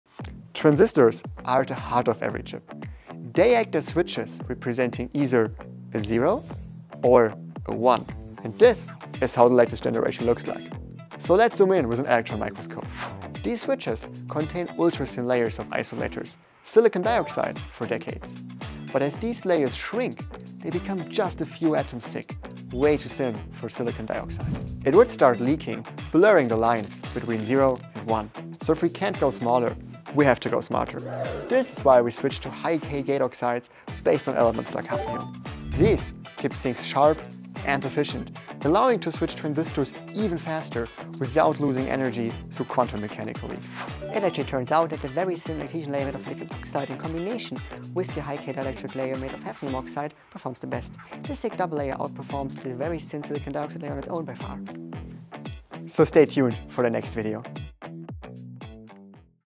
New Materials for Faster, Better Performance / 신소재, 더 빠르고 우수하게
트랜지스터는 전류를 조절하여 전자 신호와 전력을 증폭하거나 스위칭하는 핵심 반도체 소자입니다.

기존 트랜지스터는 실리콘 산화물 절연층을 사용했으나, 절연층의 두께가 점차 얇아지며 전류 누설과 성능 저하라는 한계에 부딪혔습니다.

오늘날, 차세대 반도체 혁신을 이끄는 것은 바로 스마트한 트랜지스터입니다. 이 스마트한 트랜지스터는 기존의 문제를 해결하고, 더욱 효율적이고 빠른 스위칭을 가능하게 합니다.

스마트한 트랜지스터에 대해 더 알아보시려면 영상을 확인해보세요.

Transistors are essential semiconductor devices that control current to amplify electronic signals and power, or perform switching.

Traditionally, transistors used a silicon dioxide insulating layer, but as this layer continues to thin, issues such as current leakage and performance degradation have emerged.

Today, the next wave of semiconductor innovation is driven by smarter transistors, which address these challenges and enable faster and more efficient switching.

To learn more about smart transistors, check out the video.